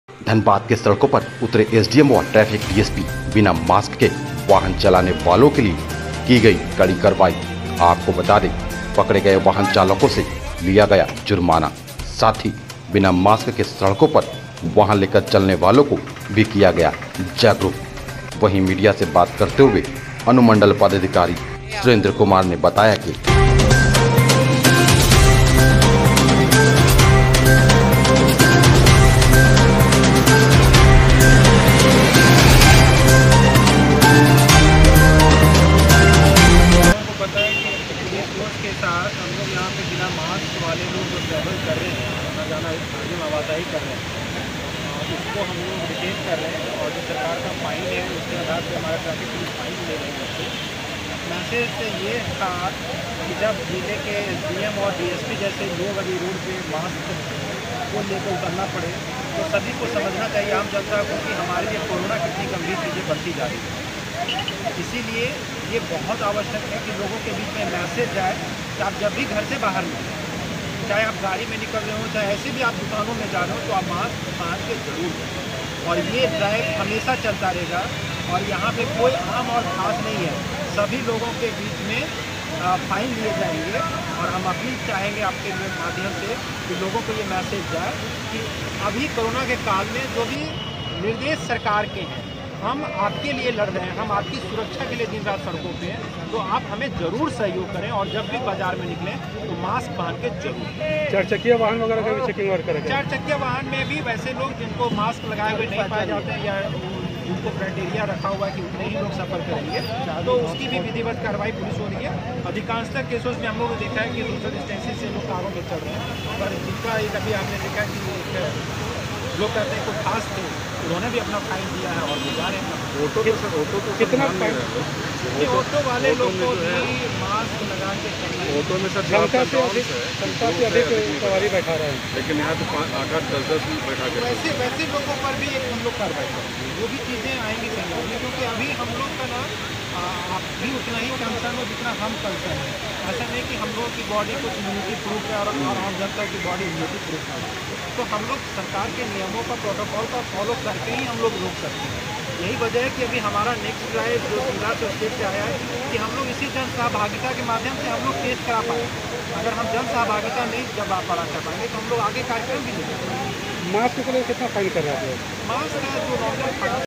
धनबाद के सड़को पर बिना मास्क के वाहन लेकर चलने वाले हो जाए सावधान:नही तो देना होगा जुर्माना
धनबाद : श्रमिक चौक पर बिना मास्क के वाहन चालक के खिलाफ रोड पर उतरे SDM और ट्रैफिक DSP  हर आने-जाने वाले वाहन को विशेष अभियान चलाकर बिना मास्क पहन कर चलाने वाले चालक पर ऑन द स्पॉट फाइन का प्रक्रिया किया जा रहा है और उन्हें हिदायत देकर पुनः इस रोड पर जब भी निकले तो मास्क पहनकर का जरूर निकले क्यों की कोरोना जैसी गंभीर बीमारी से बचने के लिए मास्क पहनना अनिवार्य है।  धनबाद अनुमंडल पदाधिकारी सुरेन्द्र कुमार ने कहा कि जब दो पदाधिकारी एसडीएम और ट्रैफिक डीएसपी खुद रोड पर उतरे हैं मास्क पहनकर तो लोगों को जागरूक होना चाहिए कि ऐसे 2 बड़े पदाधिकारी हैं जो हर चीज की सुरक्षा को ध्यान रखते हुए खुद मास्क पहनकर चलते हैं तो ऐसे में धनबाद की जनता पूरी तरह से मास्क पहनकर वाहन चलाने का आदत बना ले नहीं तो इस तरह की करवाई लगातार जारी रहेगी धनबाद की हर चौक चौराहे पर बिना मास्क वाहन चलाने वाले चालक पर करवाई किया जा रहा है।